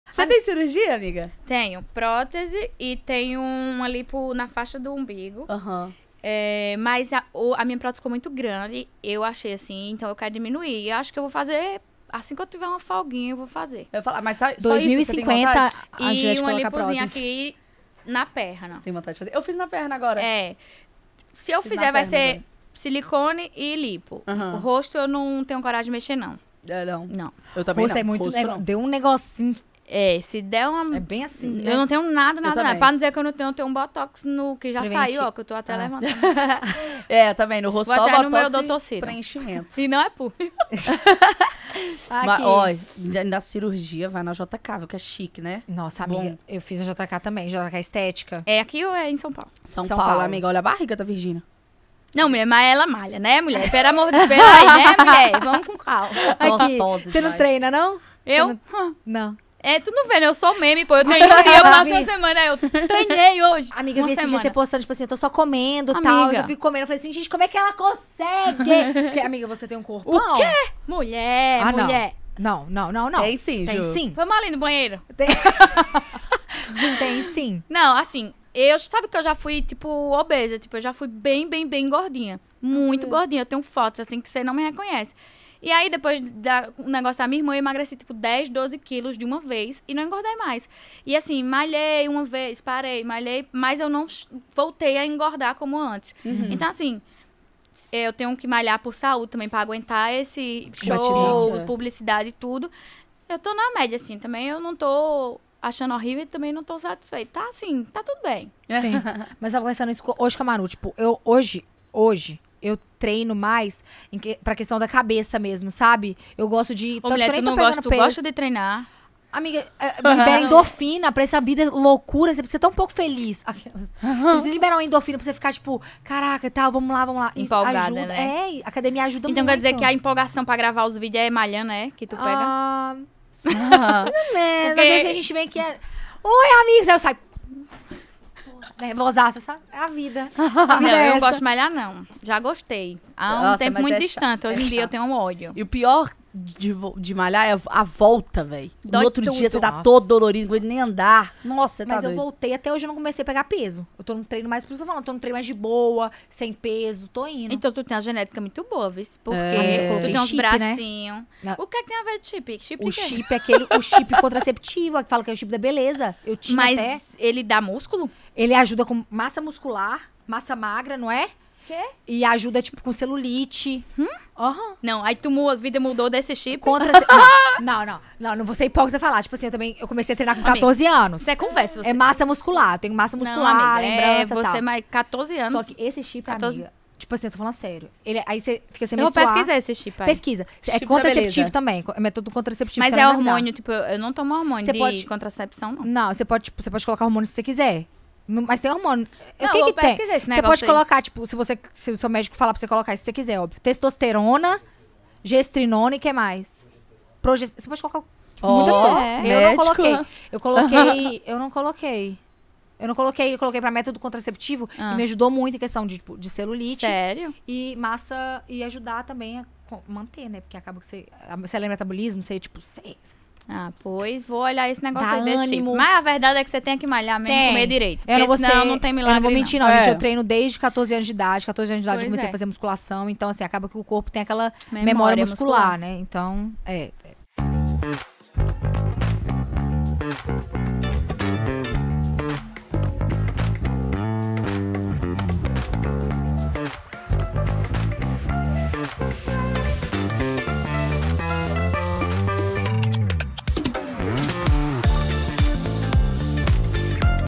QUAIS PROCEDIMENTOS ESTÉTICOS JULIETTE TEM?
E não é que chegamos na SEGUNDA TEMPORADAAAAA?

E para estrear com chave de ouro, convidamos ela que ganhou o BBB21, é cantora, empresaria e nordestina! Com vocês, Juliette Freire a rainha dos cactos 🌵😍 Tem curiosidade de saber quais procedimentos estéticos Juliette tem? Aqui nesse corte a nossa milíonaria matou as dúvidas sobre o assunto!

Fiquem ligados nesse episódio porque o cuscuz está prontooo!

✅ Instagram Oficial
CONVIDADA:
ANFITRIÃS
👩🏼Virginia: @virginia
👩🏾 Camila: @camilaloures